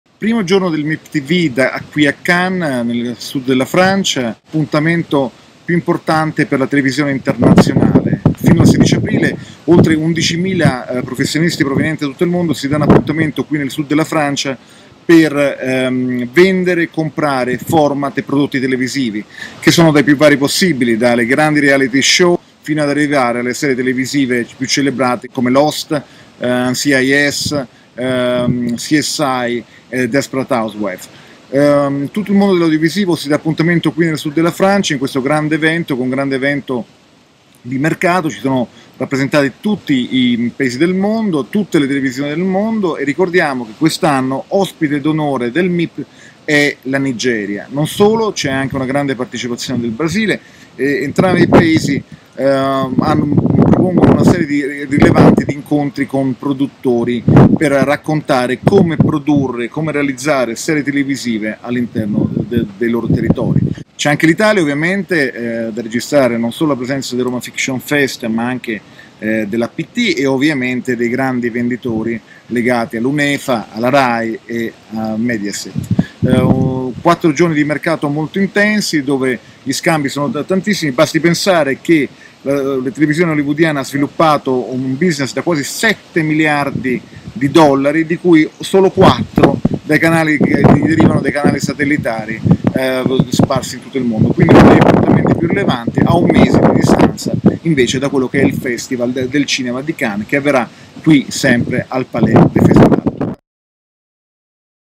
MIPTV CANNES 2010 - Giorno 1 - Primissima.it
Recensione della prima giornata MIPTV CANNES 2010. A cura di Marco Spagnoli